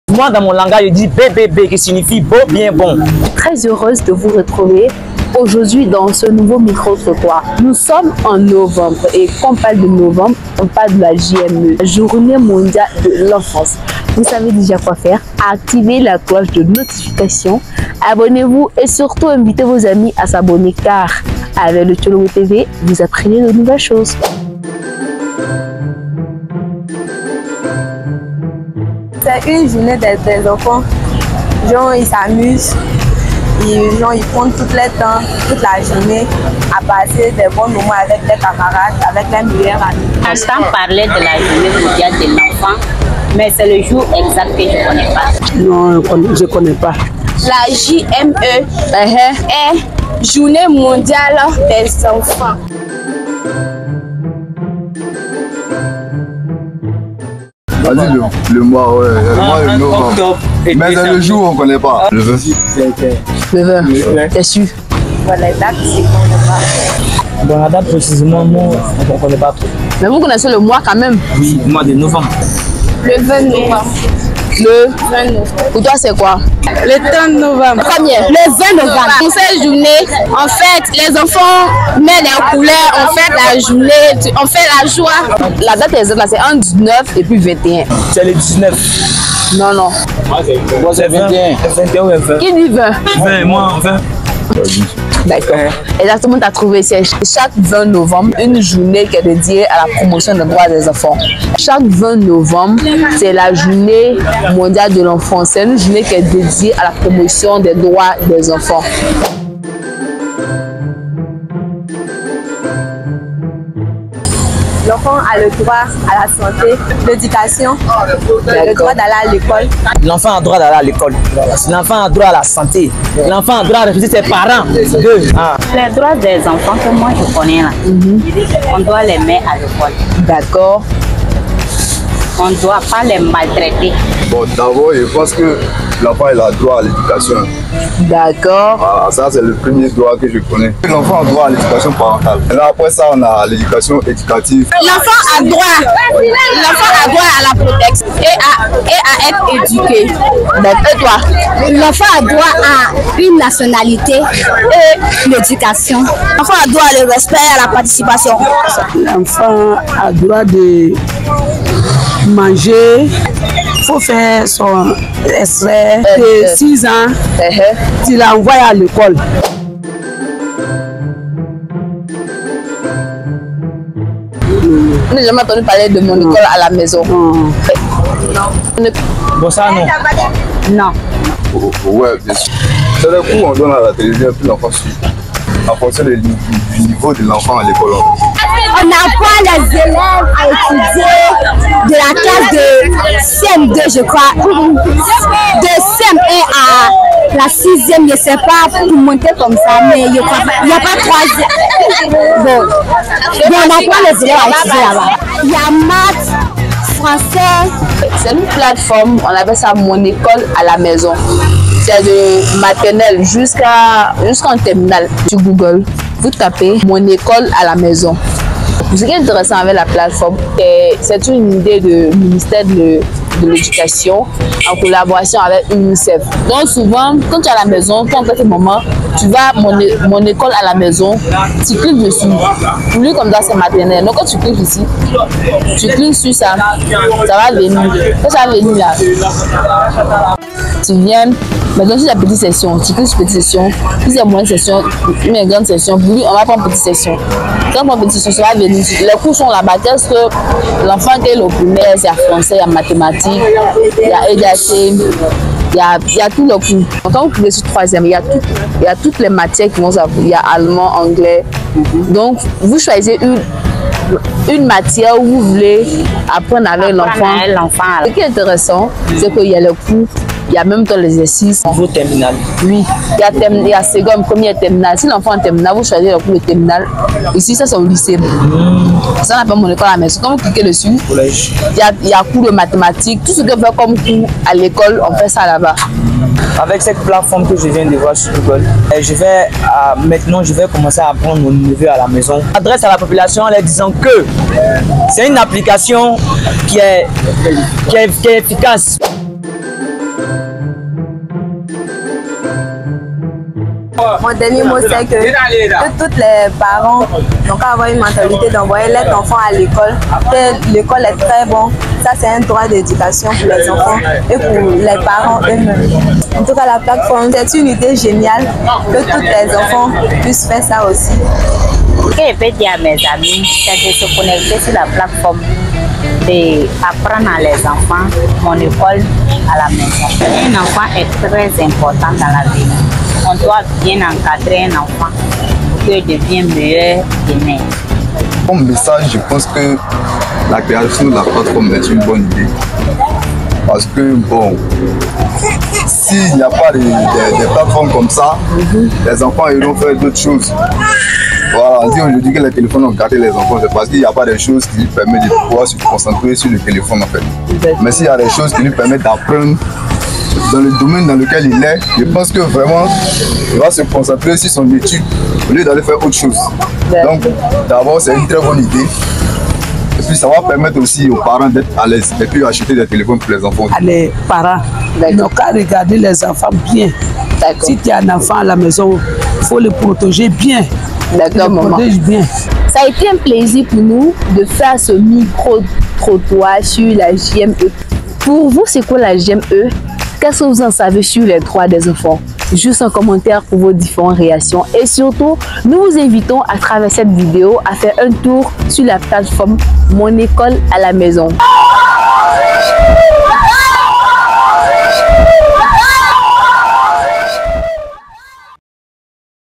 célébration de la Journée Mondiale de l'Enfance Mise en avant de la plateforme mon école à la maison
Micro trotoir réalisé dans la commune de ferkessedougou à l'occasion de la célébration de la Journée Mondiale de l'Enfance mettant en lumière la plate-forme "mon école à la maison".